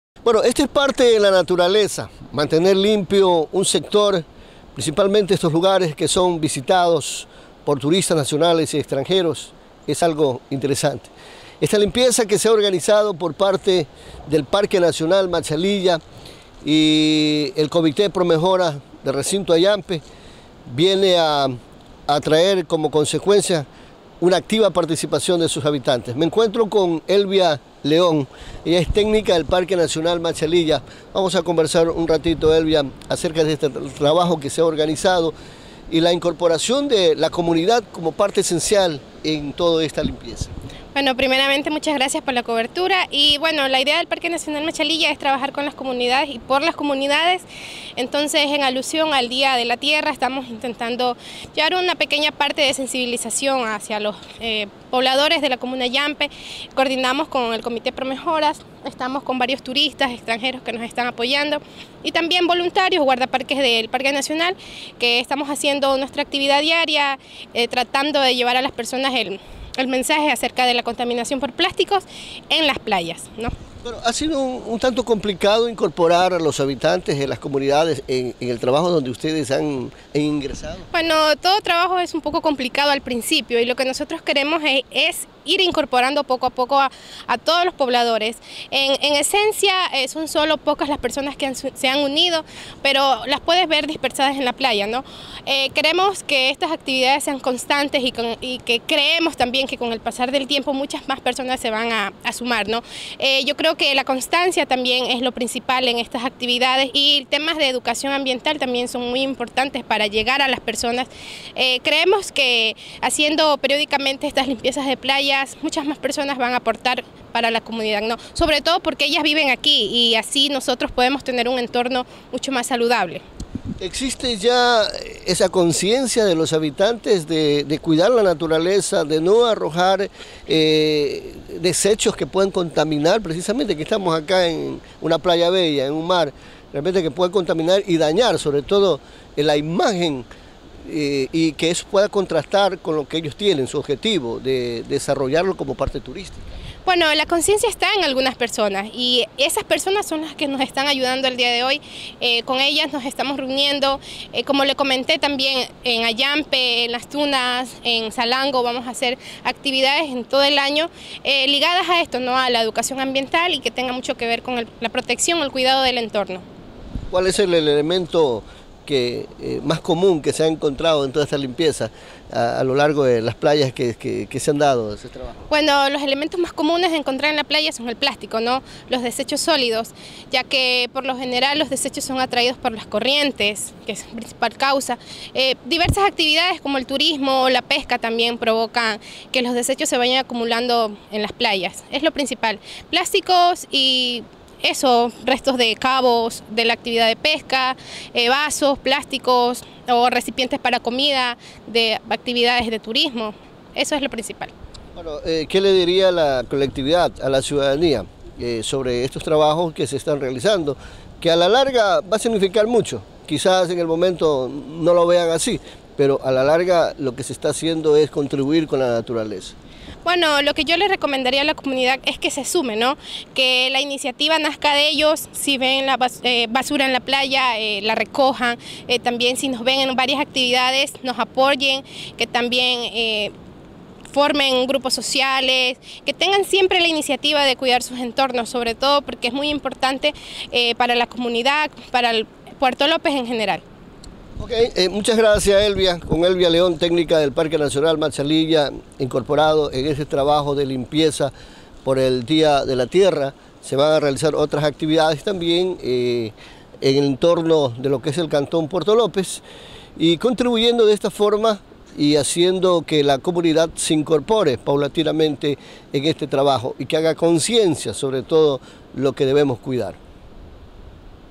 ELVIA LEÓN – TÉCNICA PARQUE NACIONAL MACHALILLA
MINGA DE LIMPIEZA POR EL DÍA DE LA TIERRA
PARQUE NACIONAL MACHALILLA – COMITÉ AYAMPE